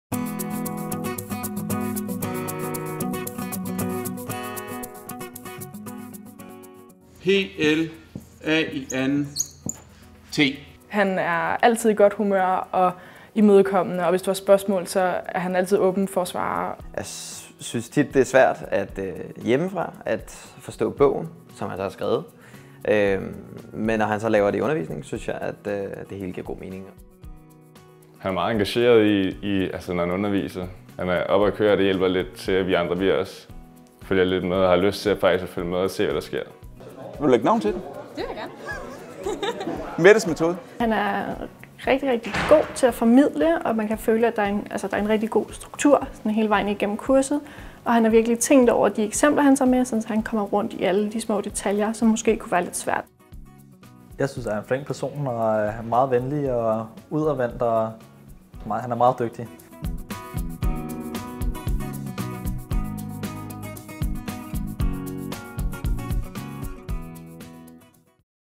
Årets underviser 2016: Jan Becker Høgsberg, DTU
Ved DTU´s Årsfest 2016 modtog lektor Jan Becker Høgsberg fra DTU Mekanik den ene af to priser som ’Årets underviser 2016’.

For at kunne uddanne ingeniører i verdensklasse skal man have undervisere i verdensklasse. Til årsfesten hyldes to af dem. De bliver nomineret af DTU´s studerende, og bliver kåret af Polyteknisk Forening (PF).